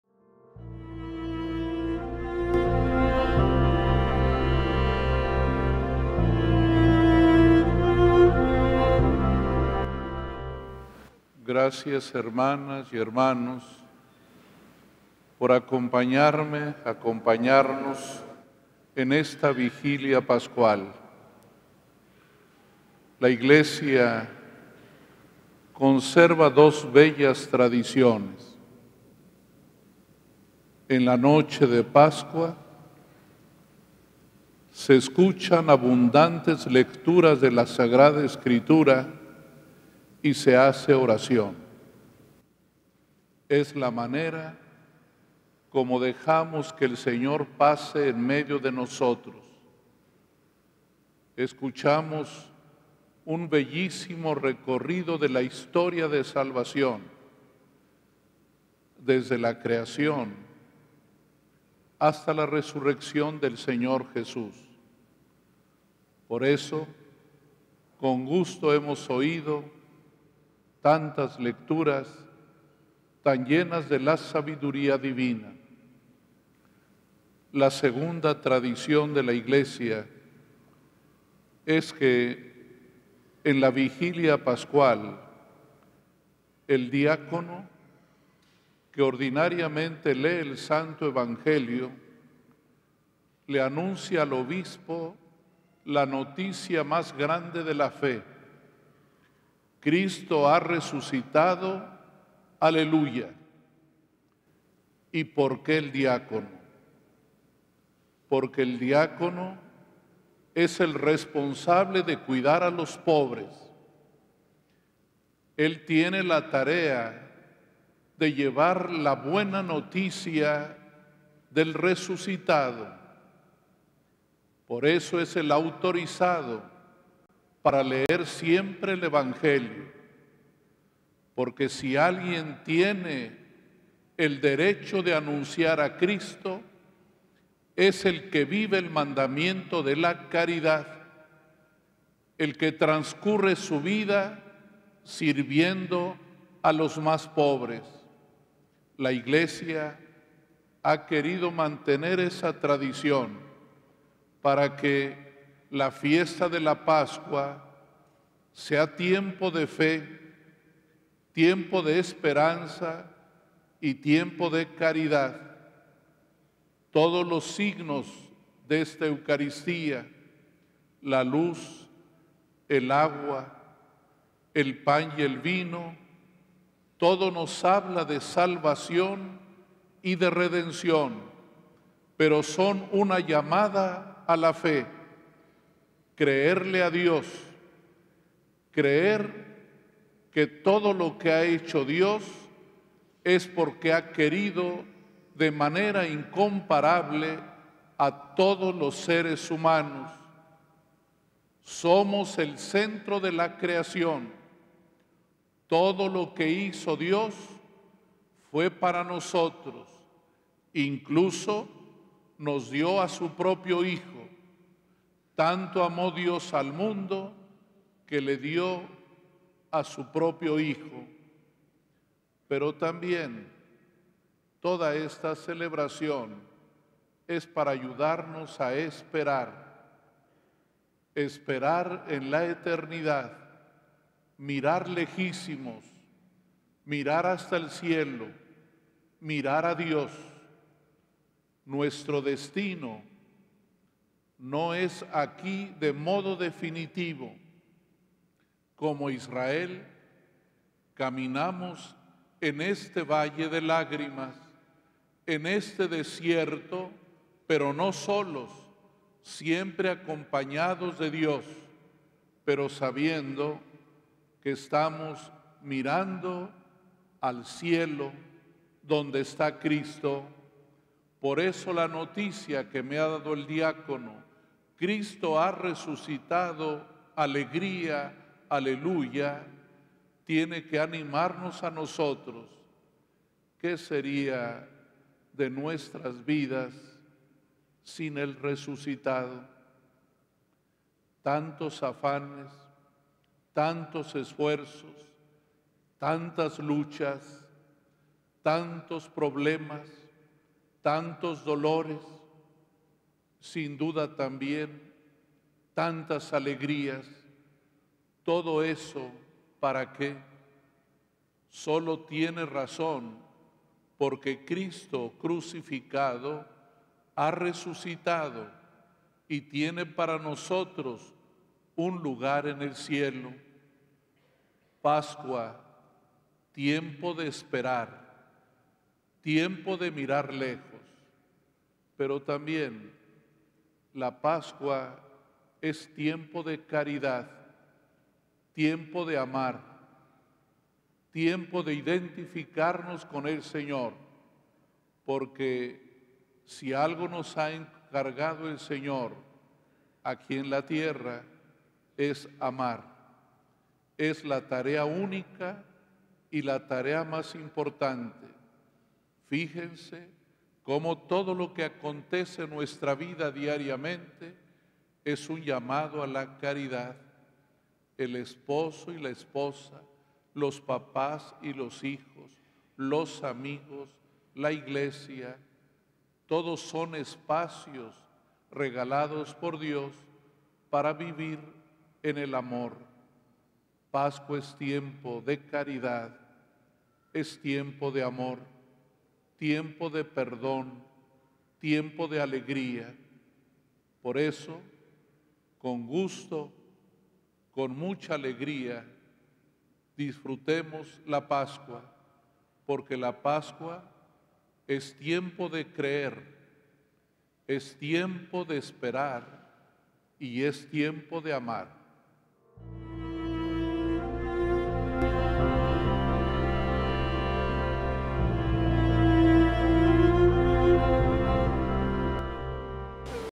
Homilía de la Vigilia Pascual del 01 de Abril del 2018
Homilía del Domingo de Ramos Ciclo B
01 de Abril del 2018
Arzobispo Mons. Rogelio Cabrera López